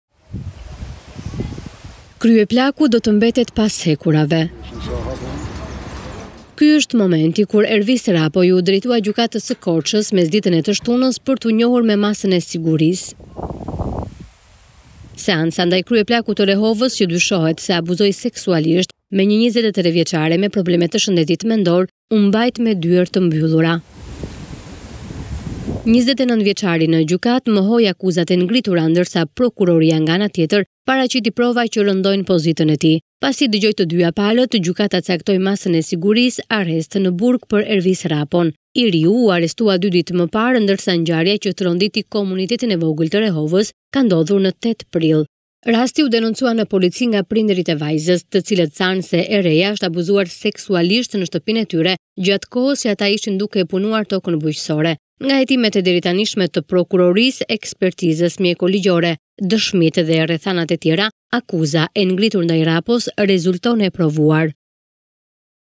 Akuzohet se përdhunoi 23 vjeçaren me probleme mendore, Gjykata lë në burg kryeplakun e Rehovës
Kryeplaku do të mbetet pas hekurave.

Ky është momenti kur Ervis Rrapo iu drejtua Gjykatës mesditën e të shtunës për t`u njohur me masën e sigurisë.

Seanca ndaj kryeplakut të Rehovës që dyshohet se abuzoi seksualisht me një 23-vjeçare me probleme të shëndetit mendor u mbajt me dyer të mbyllura.

29-vjeçari në Gjykatë mohoi akuzat e ngritura ndaj tij.

Prokuroria nga ana tjetër paraqiti prova që rëndojnë pozitën e tij.

Pasi dëgjoi të dyja palët, Gjykata caktoi masën e siguris “arrest në burg” për Ervis Rrapon.

I riu u arrestua dy ditë më parë ndërsa ngjarja që tronditi komunitetin e vogël të Rehovës ka ndodhur në 8 prill.

Rasti u denoncua në polici nga prindërit e vajzës të cilët thanë se e bija është abuzuar seksualisht në shtëpinë e tyre gjatë kohës që ata ishin duke punuar në tokë.

Nga hetimet e deri tanishme të prokurorisë, ekspertizës mjeko ligjore, dëshmitë dhe rrethanat e tjera, akuza e ngritur ndaj Rrapos rezulton e provuar.